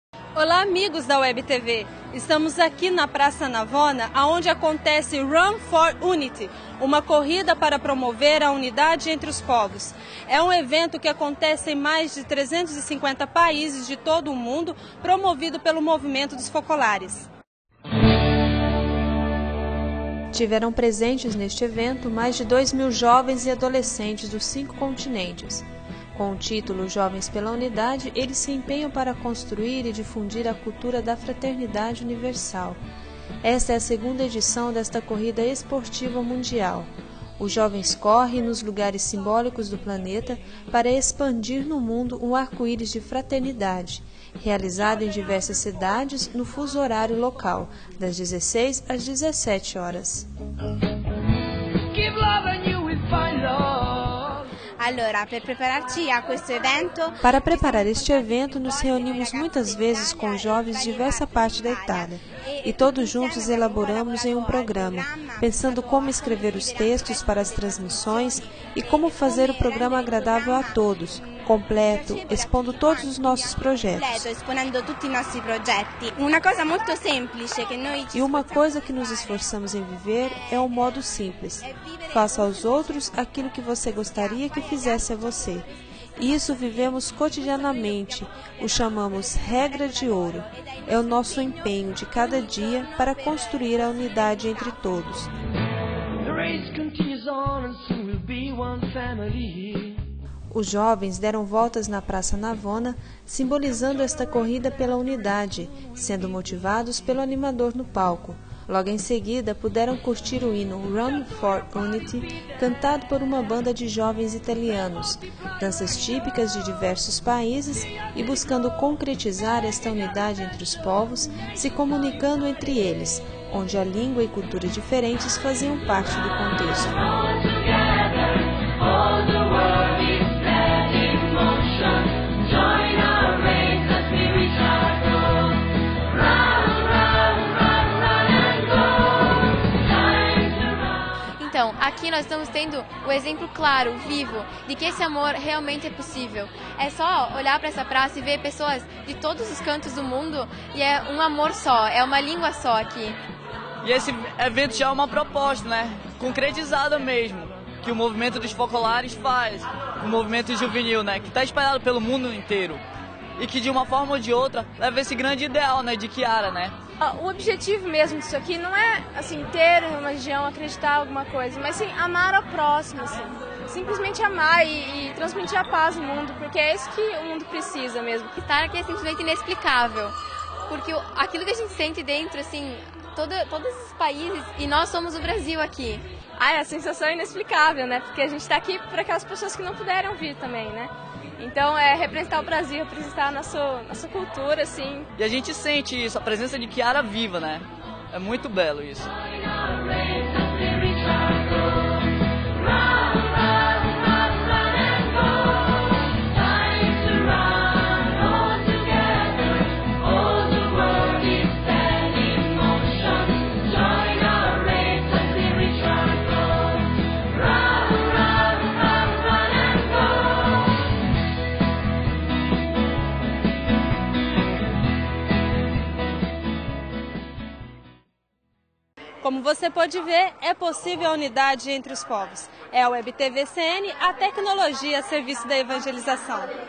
Run4Unity 2008 - Roma
A Corrida pela unidade entre os povos foi promovida pelos jovens do movimento dos focolares e foi realizado em Roma na praça Navona.